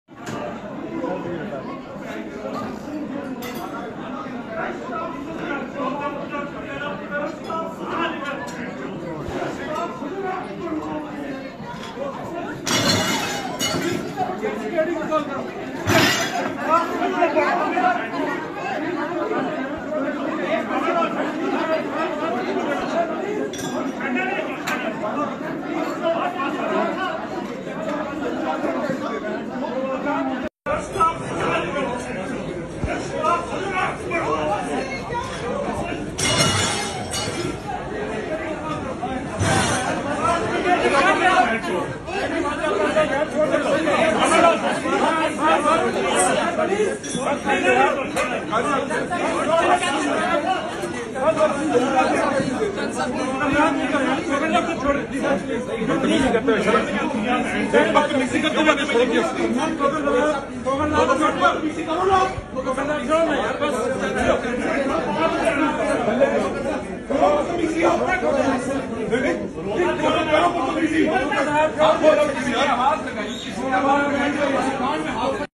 Noor Alam Khan and Mustafa Nawaz assault senior citizen | 12 April 2022
Noor Alam Khan and Mustafa Nawaz assault senior citizen
12 April 2022

ISLAMABAD: PTI estranged MNA Noor Alam Khan and PPP senator Mustafa Nawaz Khokhar were seen assaulting a citizen after he allegedly called the disgruntled PTI lawmaker, a turncoat and abused him in Pashto,
According to details, Noor Alam Khan and Mustafa Nawaz were in a local Islamabad hotel for iftar when a citizen, allegedly a PTI worker, abused them. The apparently older citizen also called Noor Alam Khan a turncoat (lota) after which things escalated.

In the video of the incident, both the leaders can be seen assaulting the citizen while other people try to stop them.

Other political leaders including Faisal Karim Kundi and Nadeem Afzal Chan can also be seen in the video.